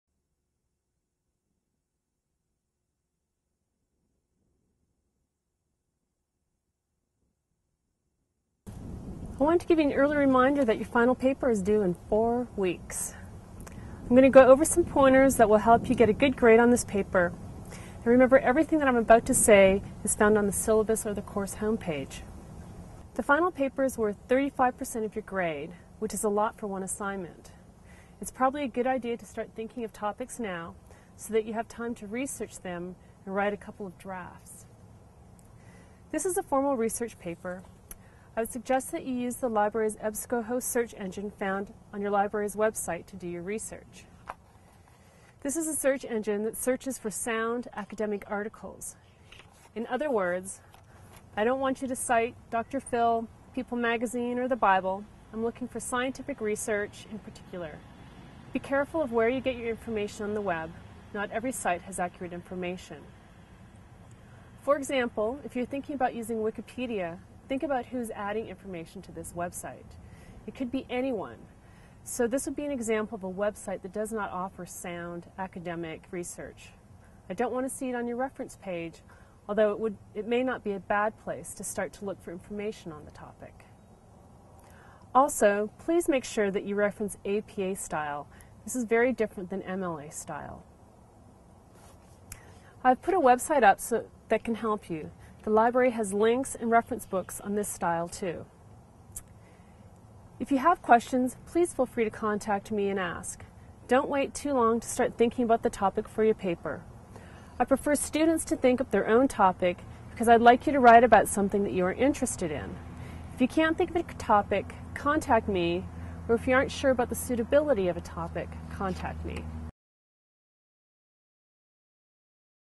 Psychology Update
A video by Kristen Kane for her online psychology students at Columbia Gorge Community College.